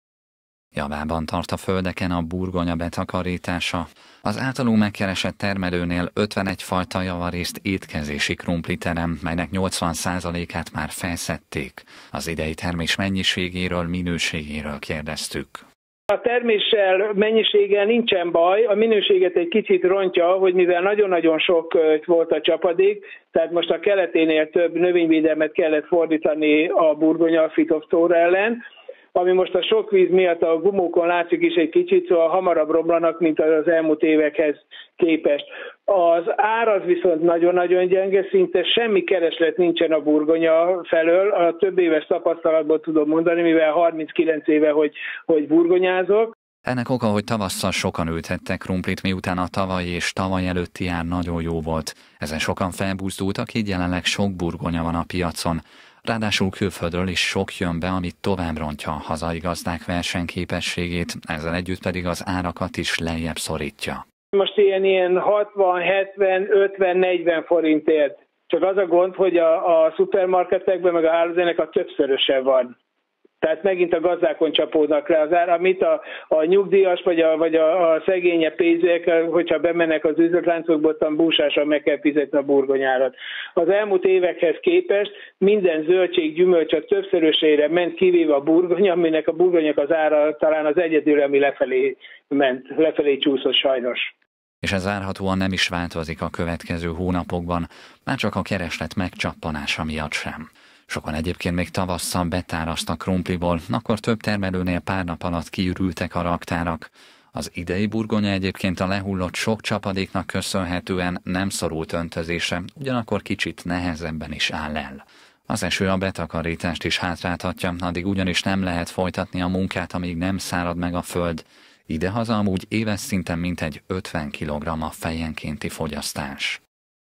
Magas terméshozam, alacsony ár - Kölcsey Tv
A mennyiséggel nem, a minőséggel már inkább volt gond az idei burgonya esetében. A termelők legkevésbé ugyanakkor az árral elégedettek, a zöldségé ugyanis a többi élelmiszernövénnyel ellentétben nem emelkedett, sőt, még csökkent is az elmúlt időszakban.